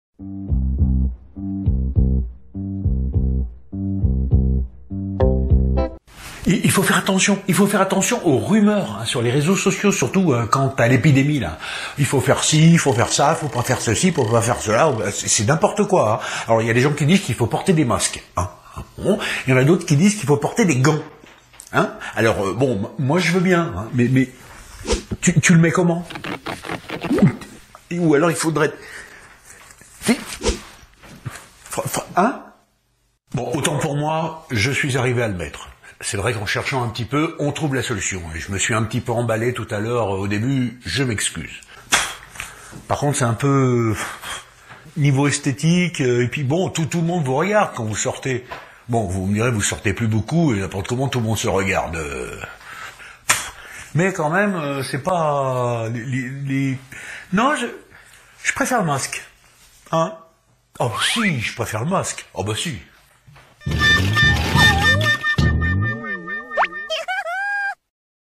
Pendant l'épidémie.Vaut-il mieux porter un masque ou des gants ?
J'ai testé pour vous  ;-)